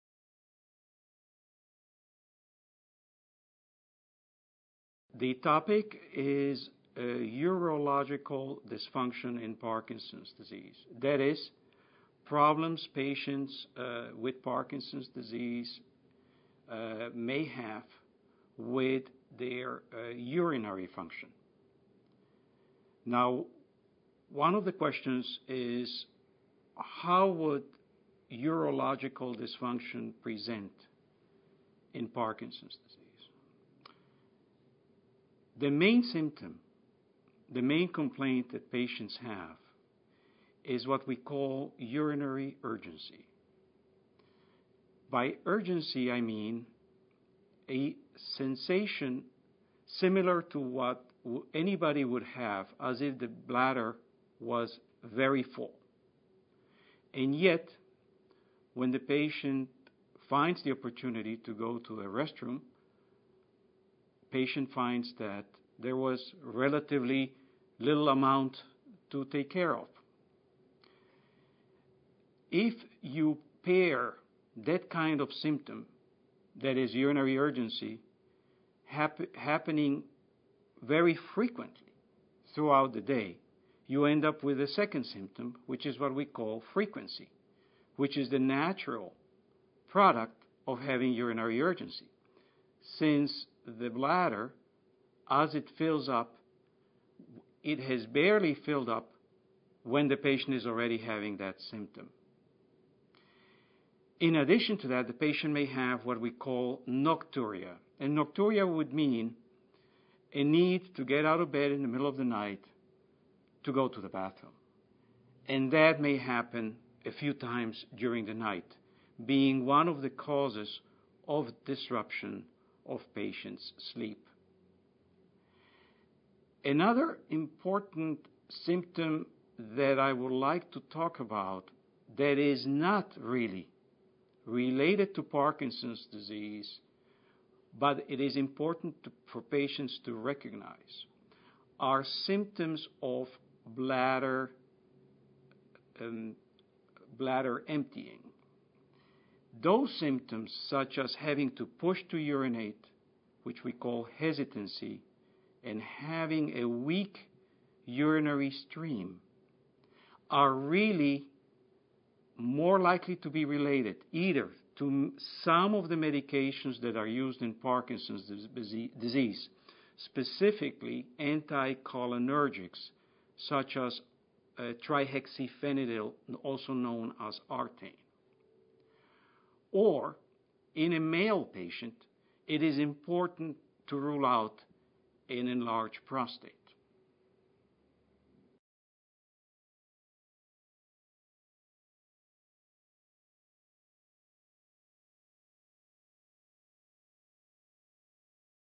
How does Parkinson's disease affect the urinary system?
National Parkinson Foundation - Carlos Singer, MD